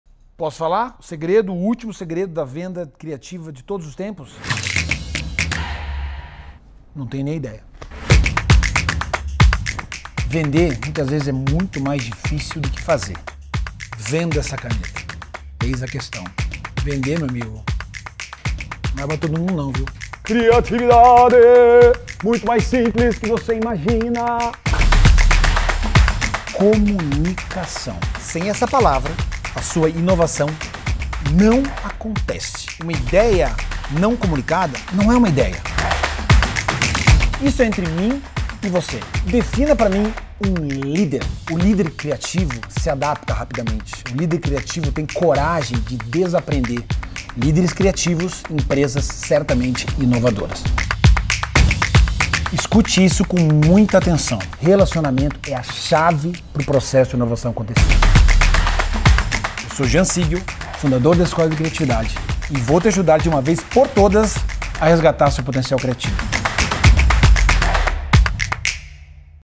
Palestras do Jean Sigel (Escola de Criatividade)
Palestra é tudo igual? Não precisa ser. Palestra com interação com o público falando sobre criatividade. Assista uma amostra do que são as palestras do Jean Sigel da Escola de Criatividade.